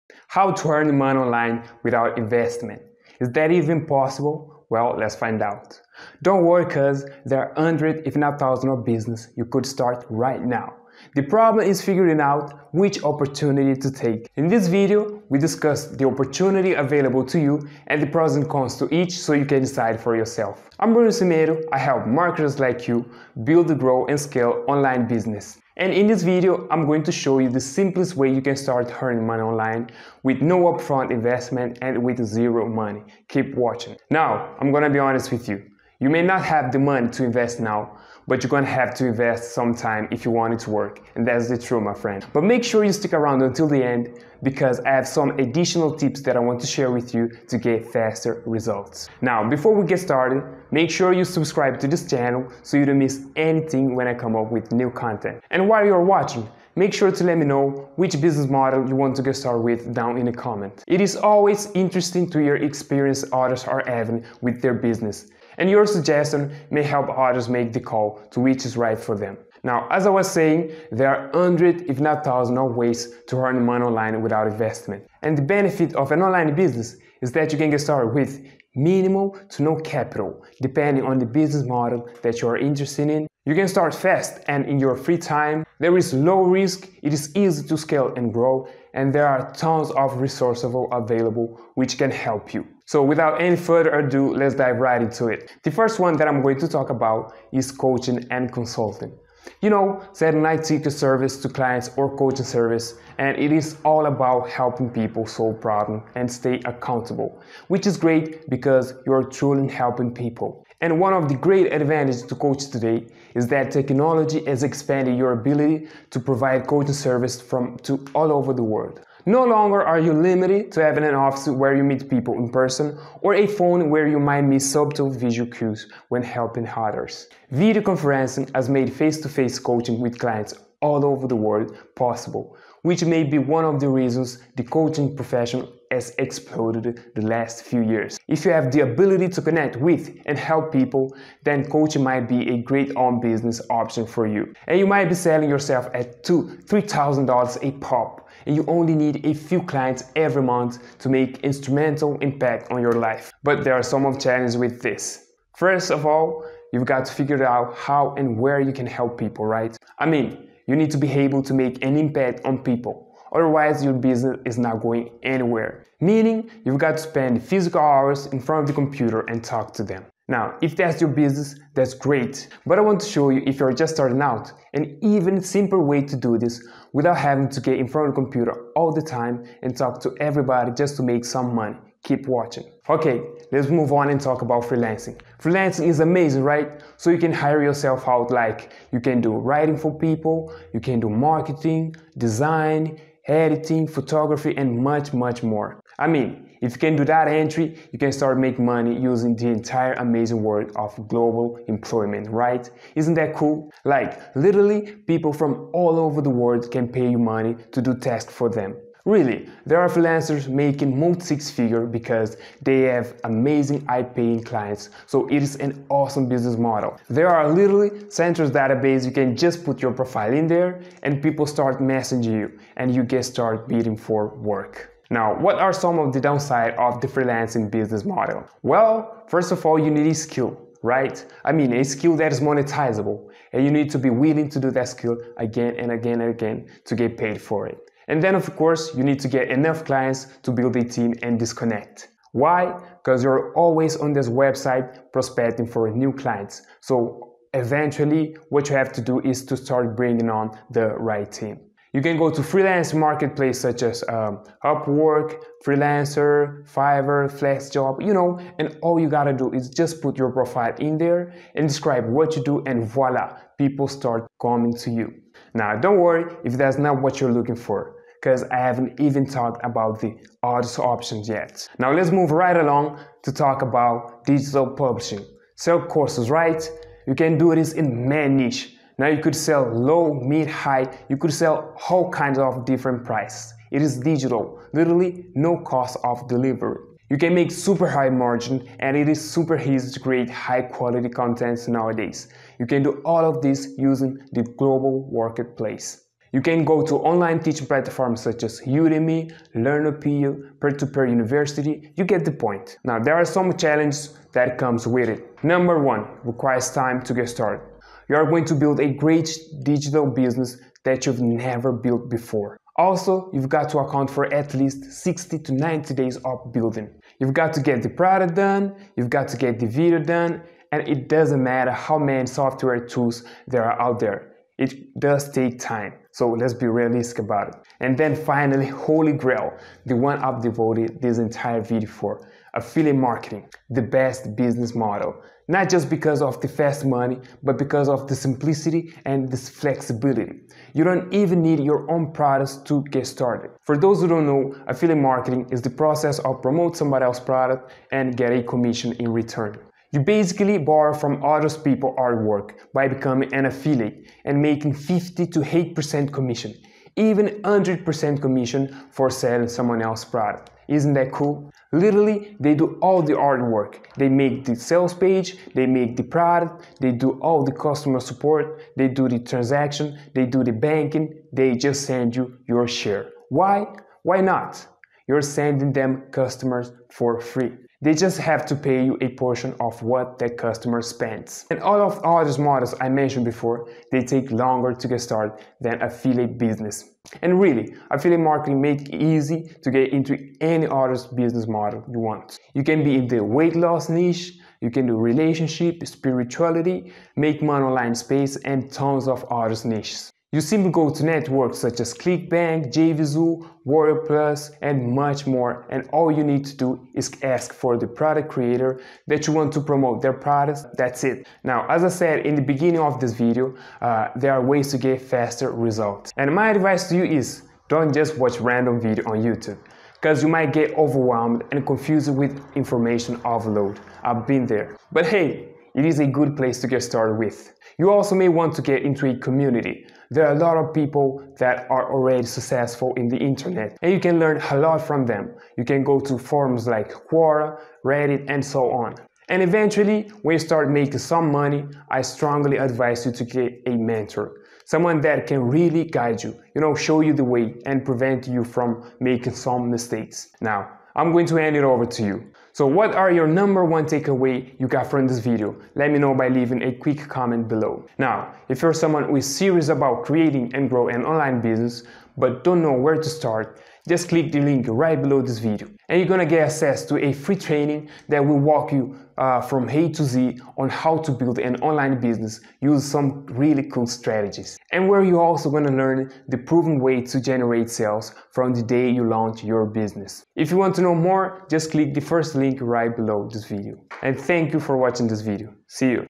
How To Earn Money Online Without Investment (Fast)
In this video I talk about the ways you can earn money online without investment and with zero money in up-front. Some requires some sort of skills others you don't even need to have any experience.

With the internet and technology today you can easily start an online business, and better yet, you can earn passive income.

I really hope that you enjoy the video and get some value from it.